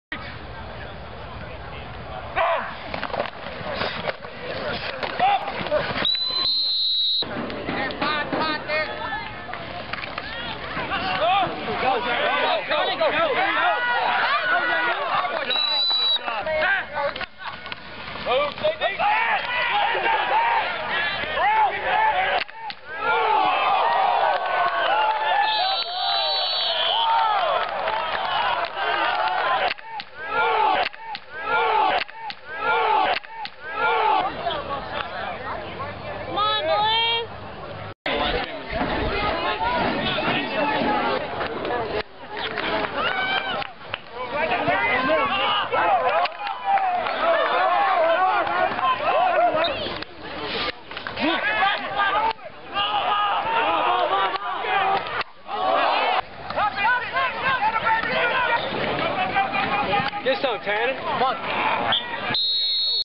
Rockets football scrimmage
Crittenden County beat Muhlenberg South 3-2 in a football scrimmage Friday, Aug. 15, 2008 at Marion, Kentucky's Rocket Stadium.